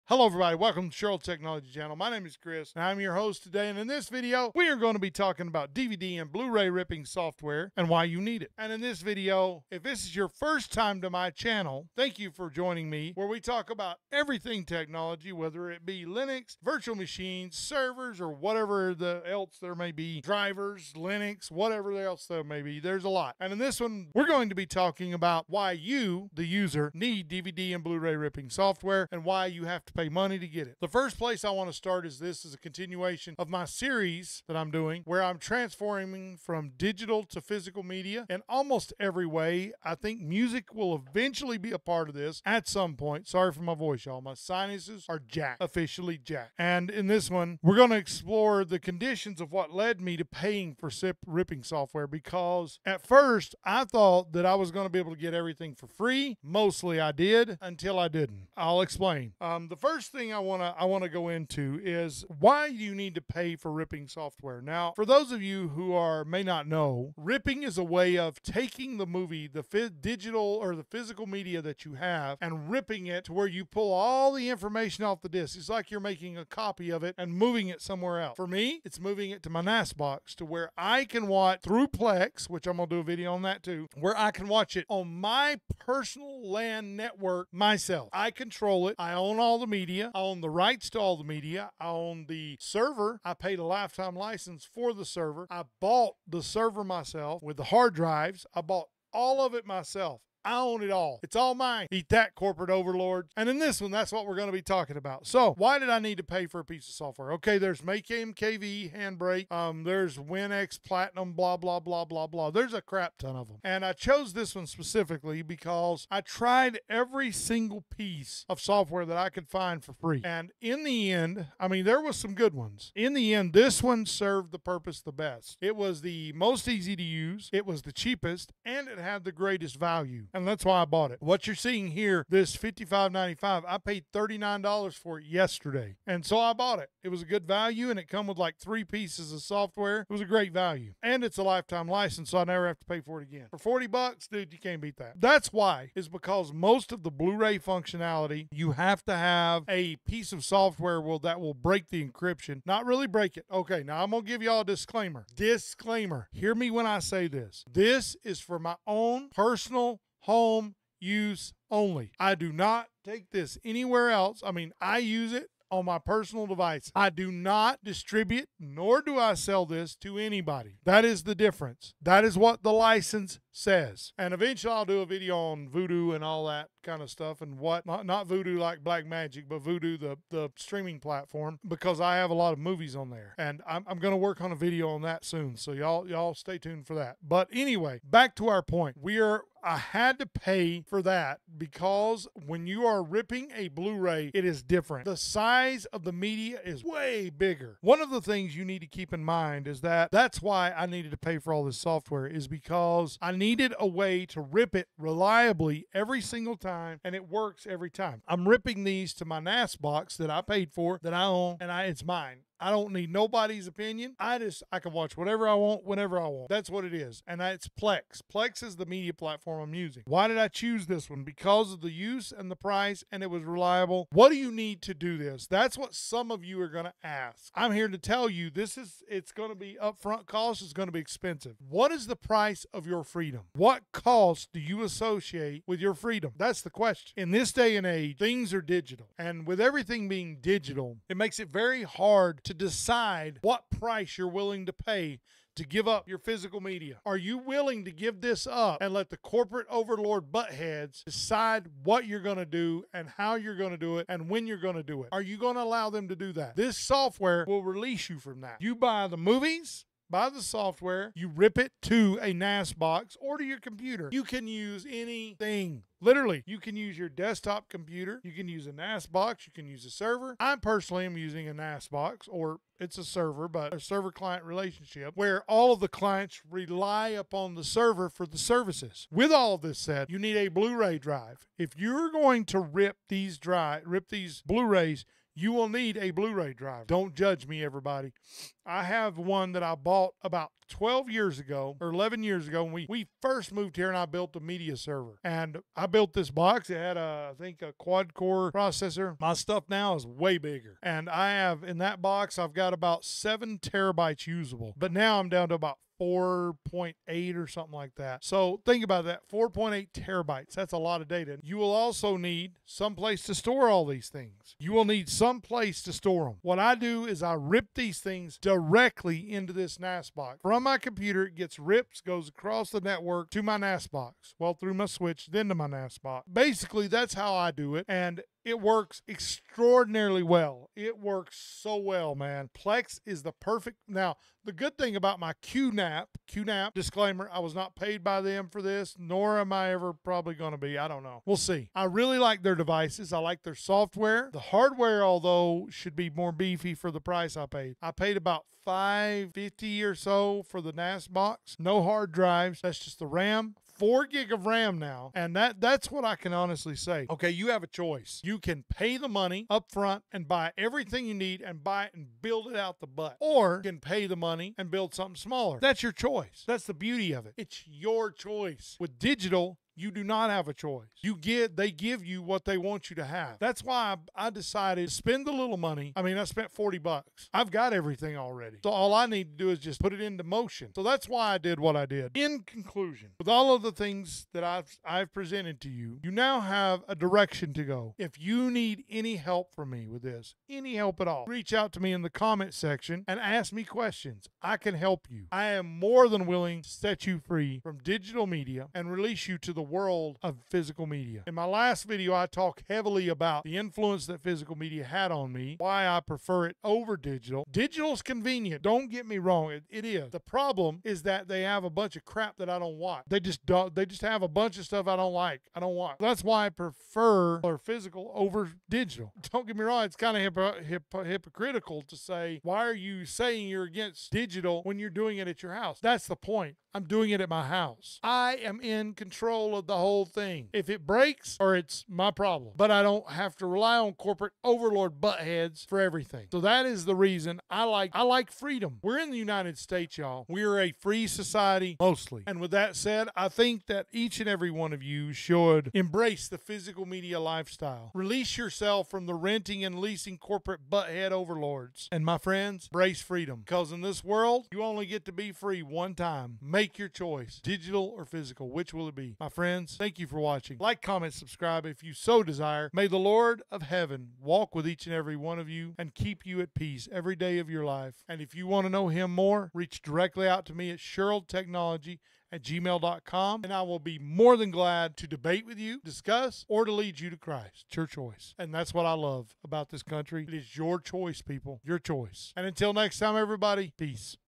DVD and Bluray Ripping Software
In this one we examine different options for Blu ray and DVD ripping. I hope all of you enjoy this video!